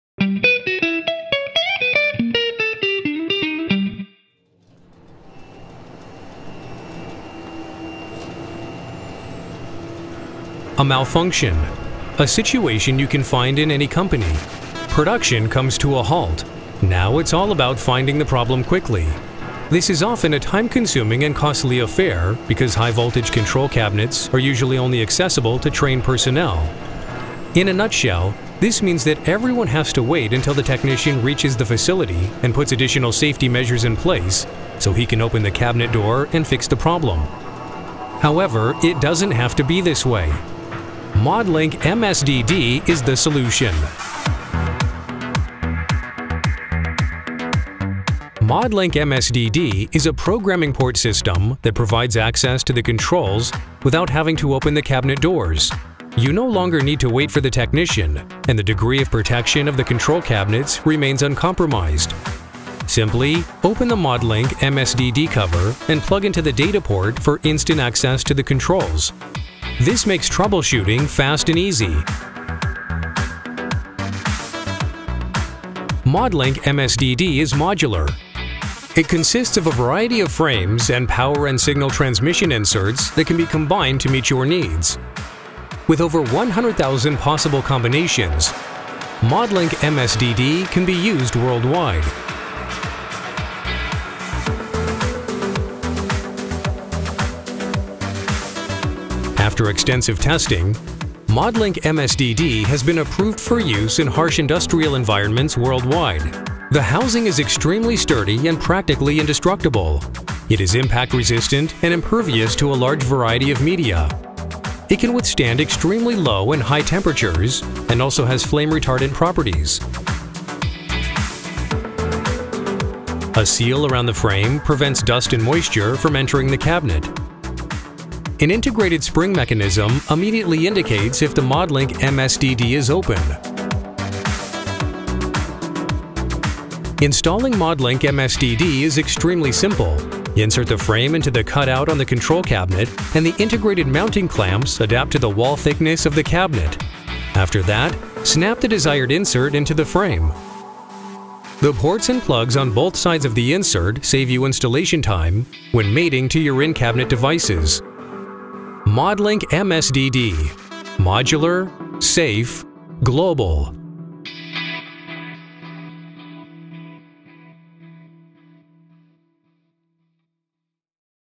Modlink MSDD - Front panel interface
Modlink MSDD front panel interfaces make it easy to access controls in the cabinet when it's time for on-site diagnostics and maintenance. The control cabinet stays closed so you aren’t exposed to live voltage and the components stay within the recommended protection class. Compliance with safety regulations regarding panel entry are easier to meet with the use of panel interface devices.

Modlink MSDD is a modular system with a large range of components and over 100,000 combination options. It's designed for worldwide applications with countryspecific models and approvals like cURus.